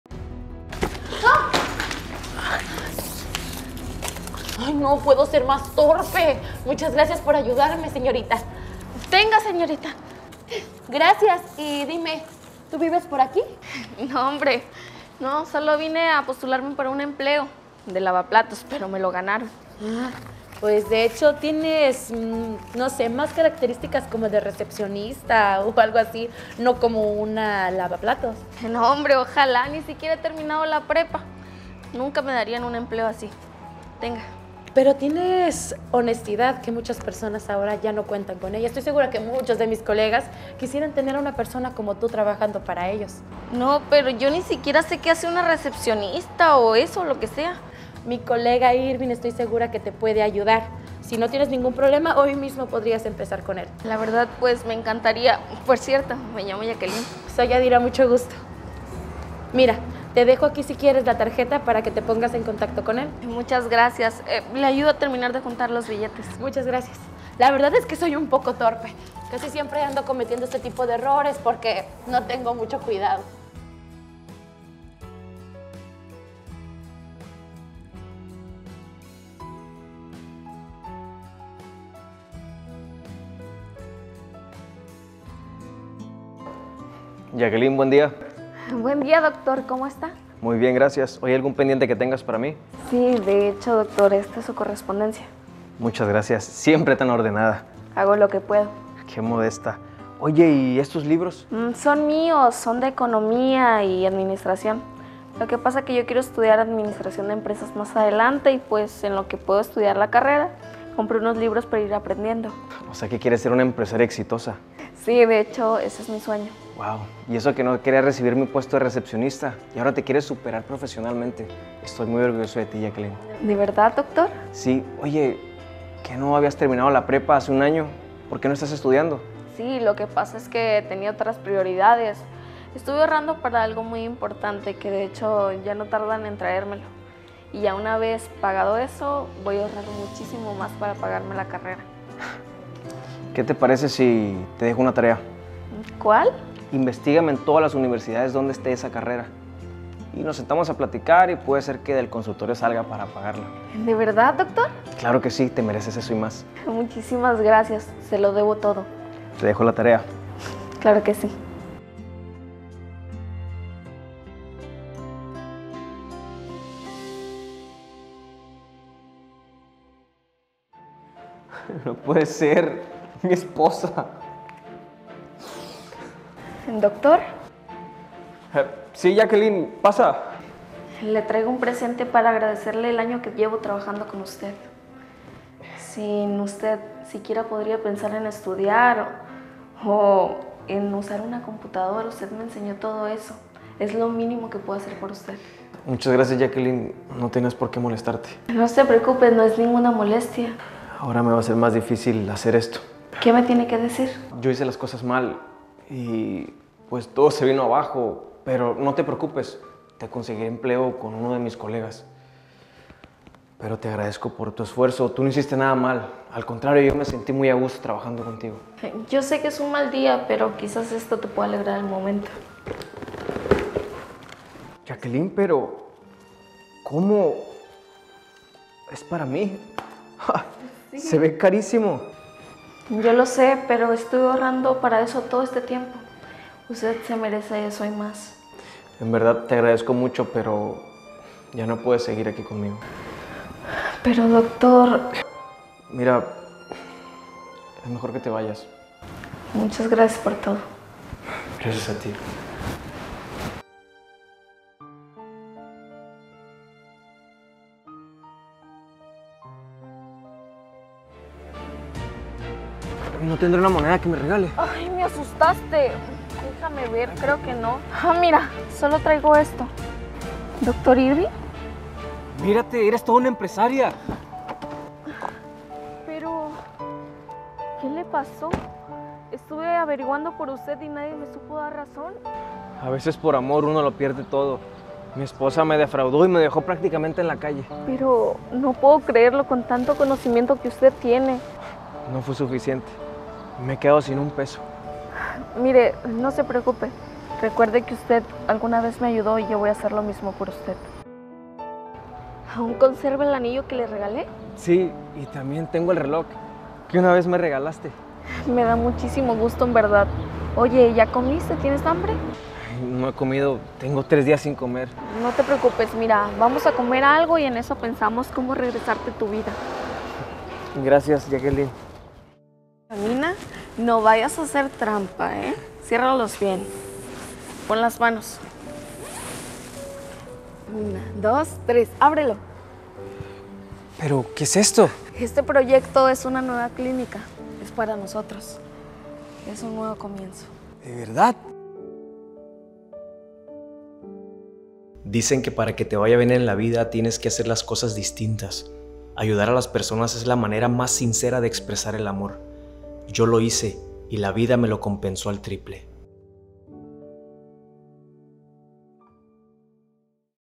Este médico ayudó a una mujer de la calle y la vida lo recompensó.
El cumplió su labor como médico y ayudó a una mujer de la calle y la vida lo recompensó...